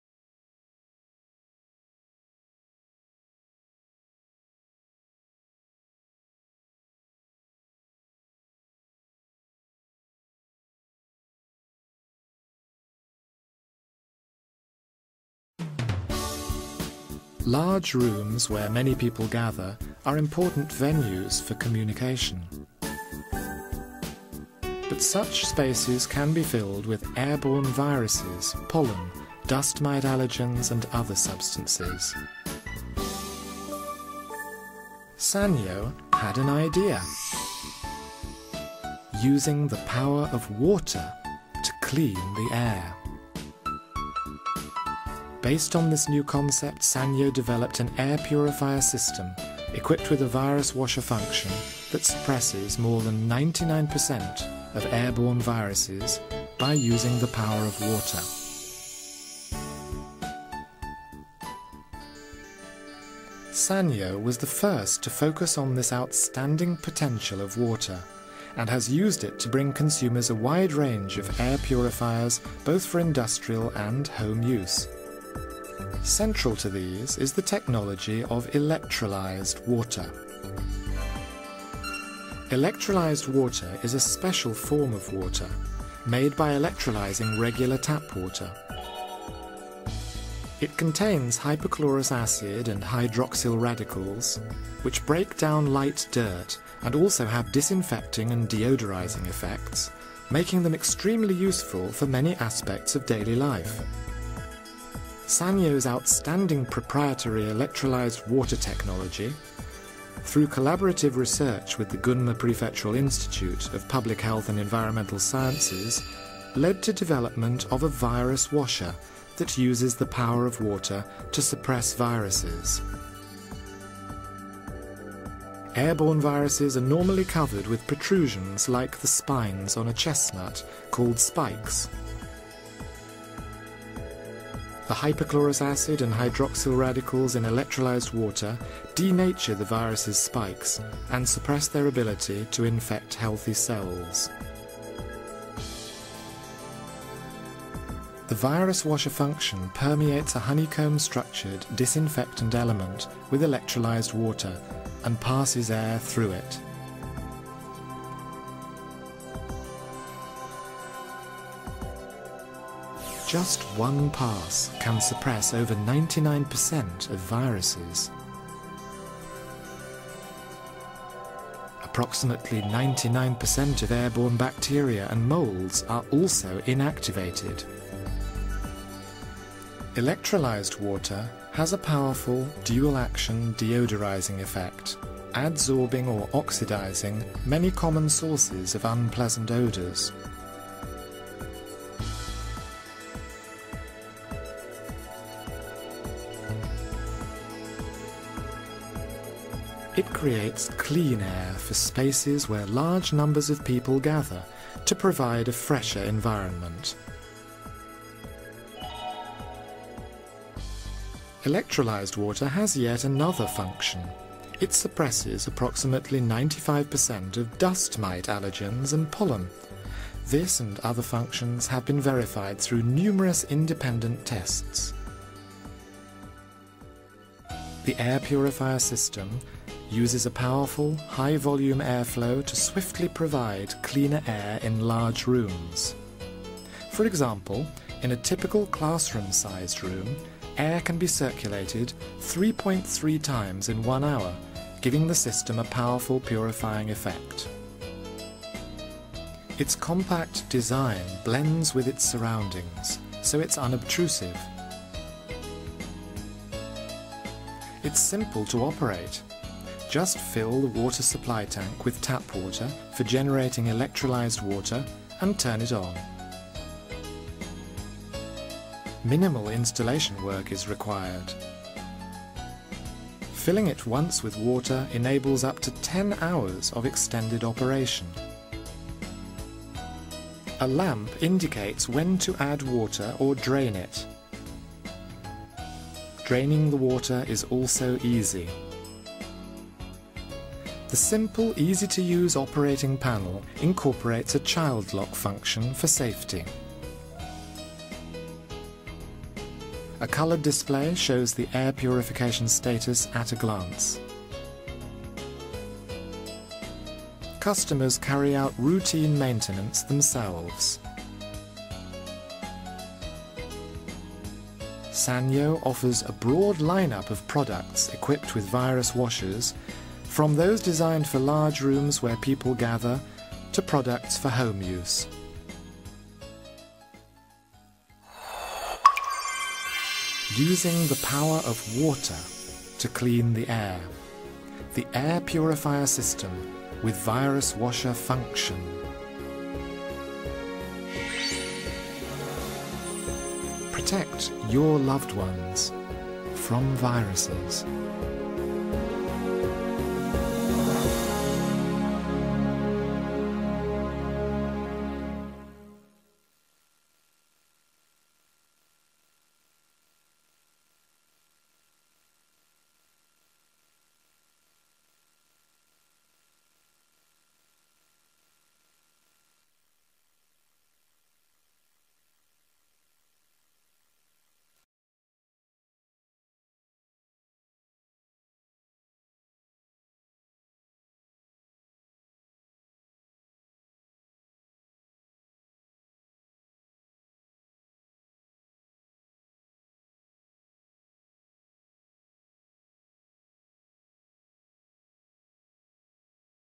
VW-VF10BG: Protect Your Loved Ones from Swine Flu with the Sanyo Virus Washer
The VW-VF10BG Virus Washer has been specifically designed to suppress airborne contaminants including H1N1 Influenza (Swine Flu) in open plan areas up to 100m2 such as in larger offices & health centres.